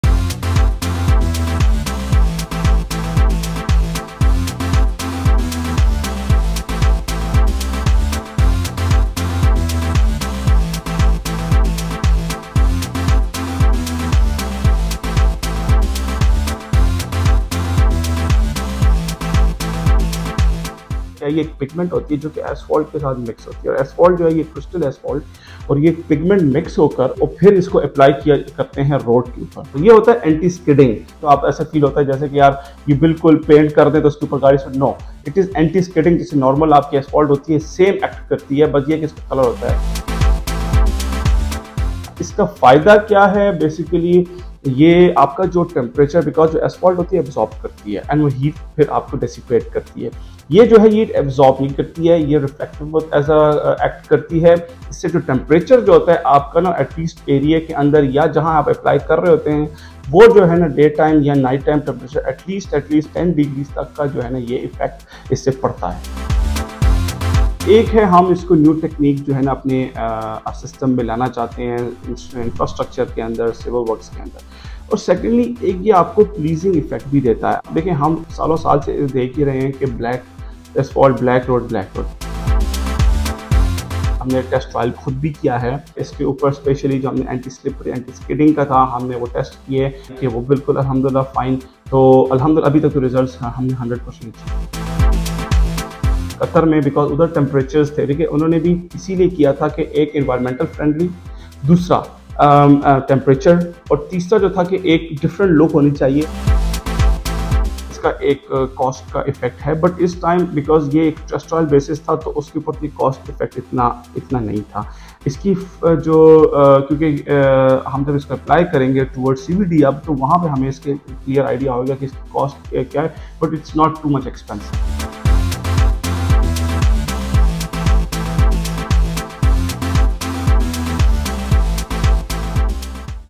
لاہور میں بنایا گیا پاکستان کا پہلا ’بلیو روڈ‘، اس کا فائدہ کیا ہے؟
لاہور میں بنایا گیا پاکستان کا پہلا ’بلیو روڈ‘، اس کا فائدہ کیا ہے اور عام سڑکوں سے مختلف کیوں ہے؟